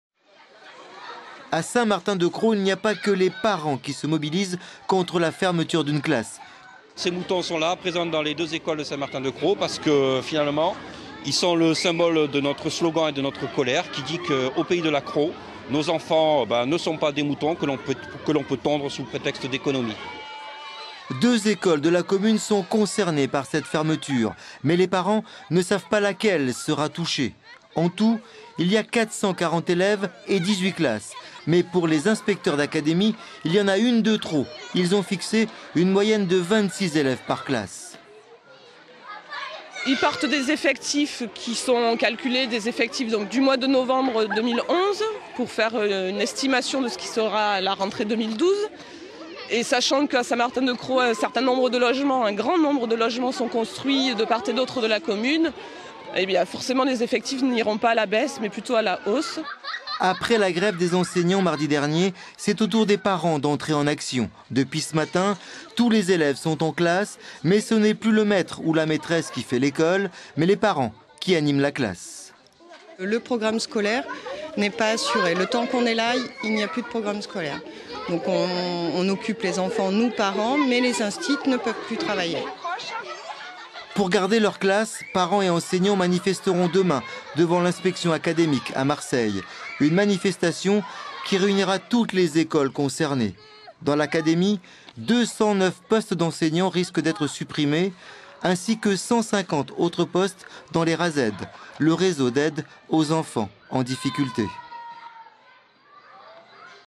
Des Moutons occupent une école à St Martin de Crau
Une classe doit disparaitre à St Martin de Crau en septembre prochain.
Là aussi, comme à Marseille hier, les parents d'élèves réagissent, ils occupent les locaux.
Et pour exprimer leur mécontentement, ils ont choisi les grands moyens.
Reportage de Robert Papin et Francis di Cesare